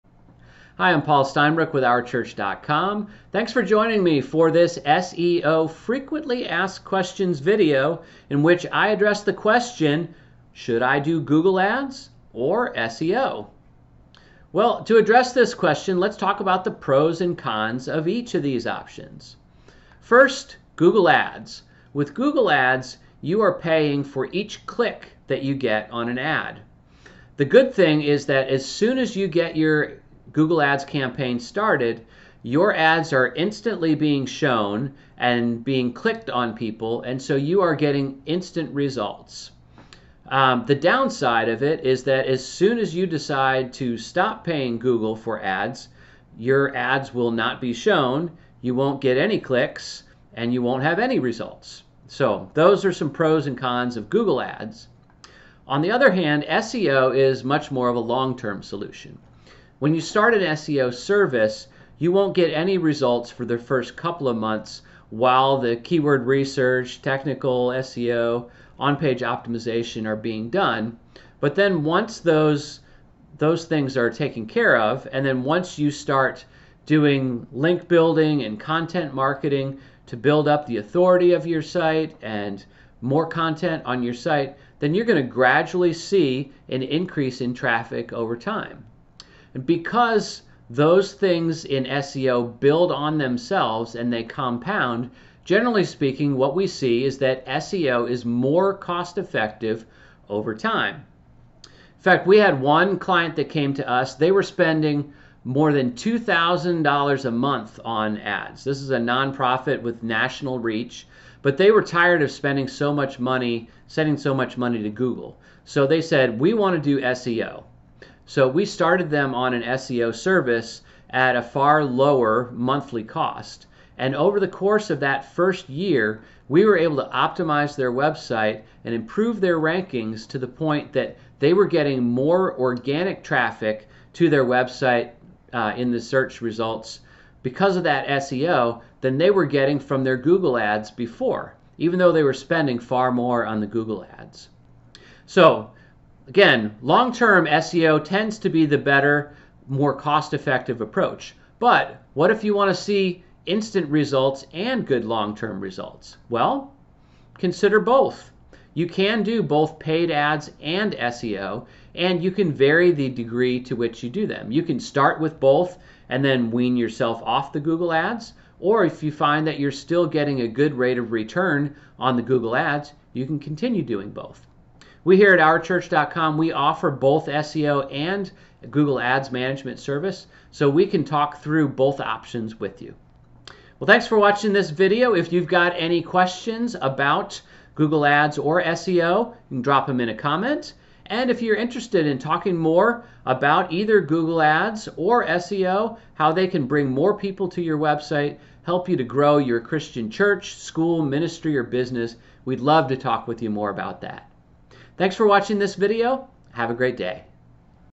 Should I do Google Ads or SEO?
SEO FAQ #15: Which is better - Google Ads or search engine optimization? It depends. :) Here's how to decide whether you should do Google Ads, SEO or both.